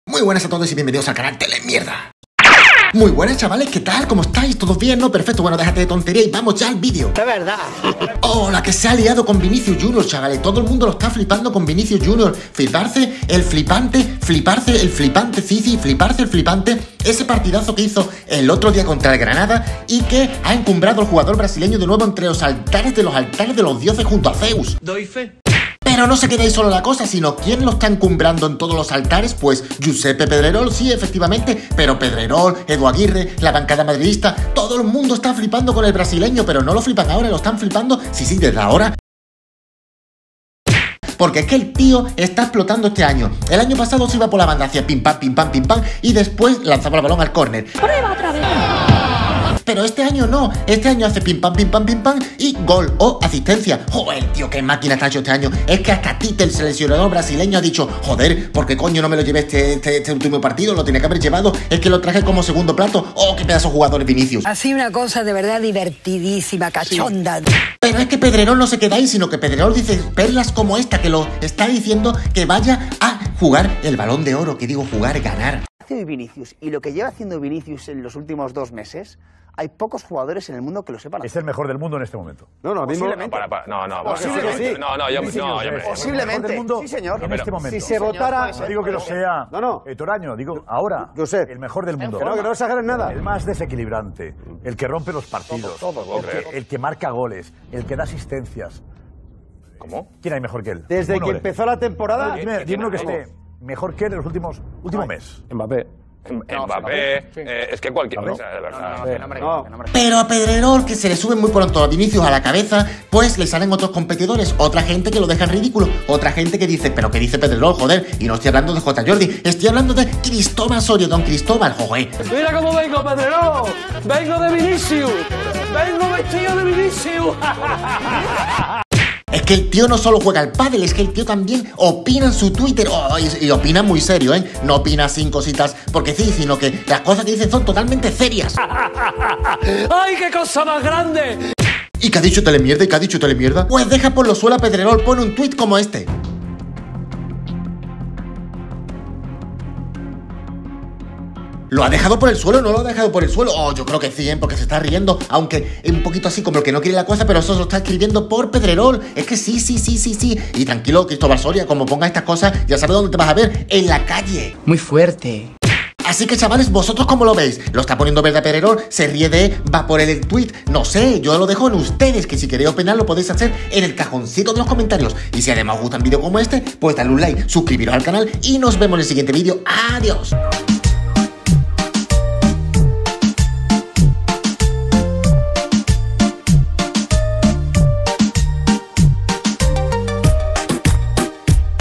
CRISTOBAL SORIA SE RIE DE PEDREROL Y SE JUEGA EL PUESTO
Cristobal Soria le echa bemoles y se juega el puesto de trabajo riendose de Pedrerol en twitter.